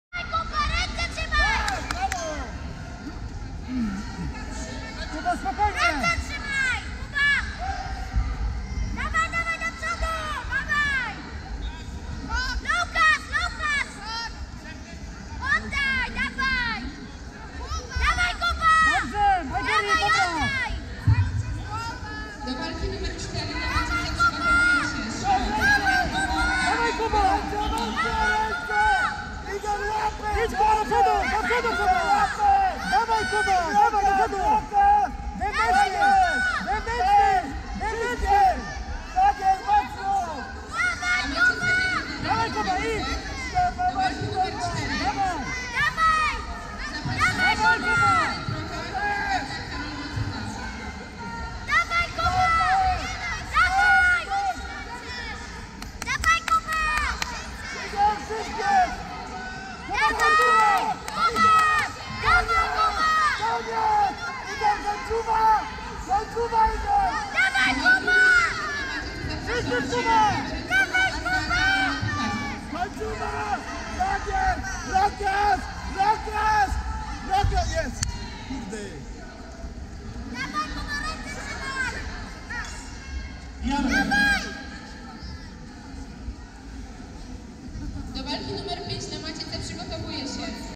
Igor Jarosz biały
V Ogólnopolski Turniej Karate Kyokushin "Gwarek Cup" o Puchar Burmistrza Libiąża - Libiąż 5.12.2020 r.